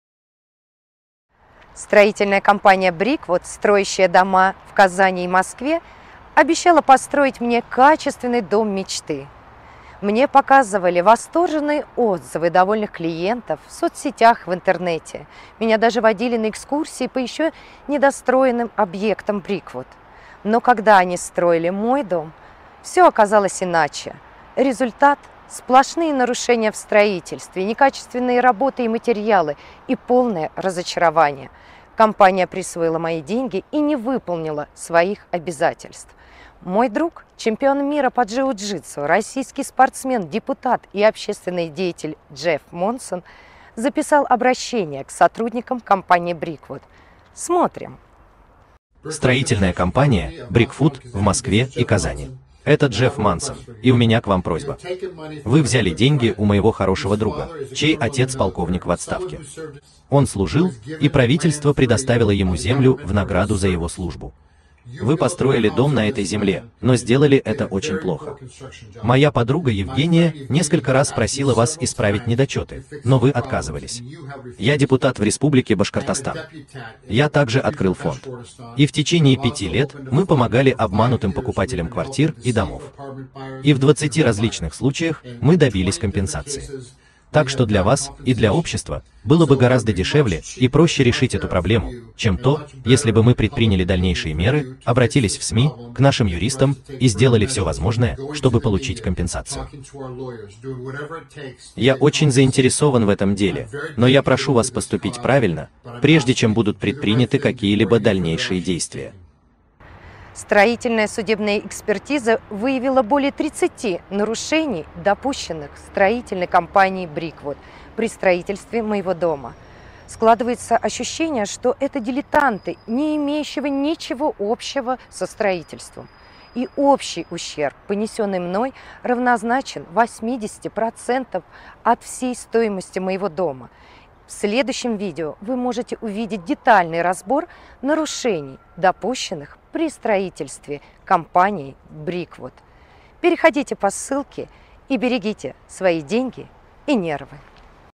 Джефф Монсон предупреждает СК "Бриквуд"
Если вы пострадали от действий компании Бриквуд (независимо от размера ущерба и местоположения— Бриквуд Москва или Бриквуд Казань), пишите на почту . Мы объединим усилия для подачи коллективного иска в суд и жалобы в органы власти.

Это видео с обращением чемпиона мира по  джиу-джитсу, российского спортсмена, депутата и общественного деятеля Джеффа Монсона к строительной компании "Бриквуд", работающей в Казане и Москве. От их деятельности пострадали люди, которые заказывали у них строительство домов. Ужасное качество, обман, вытаскивание денег из клиентов, хамство- это всё о строительной компании "Бриквуд". И всё это во время важнейшего периода жизни нашей страны, когда необходимо проявлять доблесть, совесть и мужество, а не крысятничество! Пришла пора положить конец этой вседозволенности и бесчестию. Вместе мы сила!